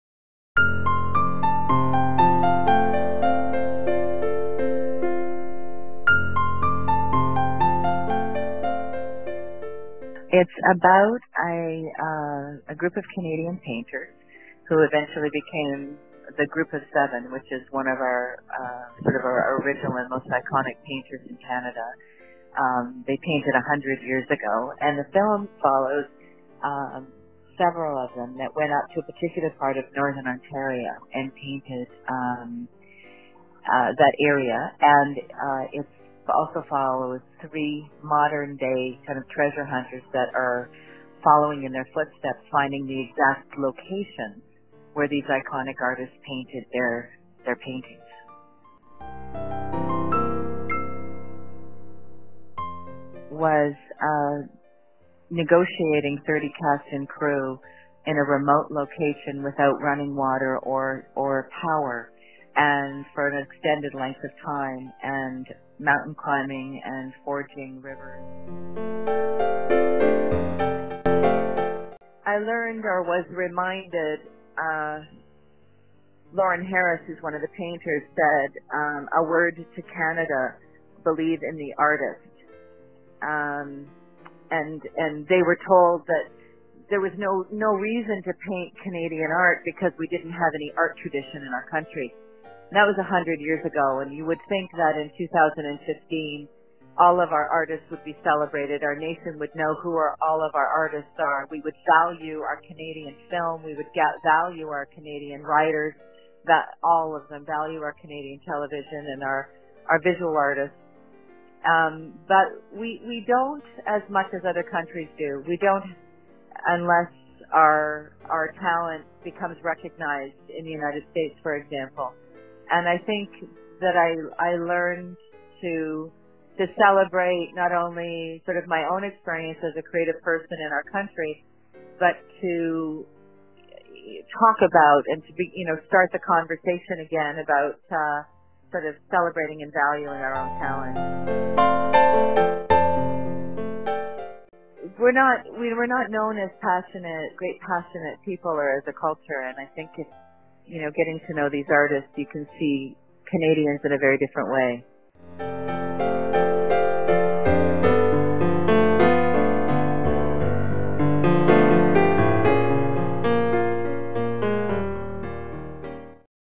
Phyllis Ellis talks about Painted Land: In Search of the Group of Seven
Trayecto F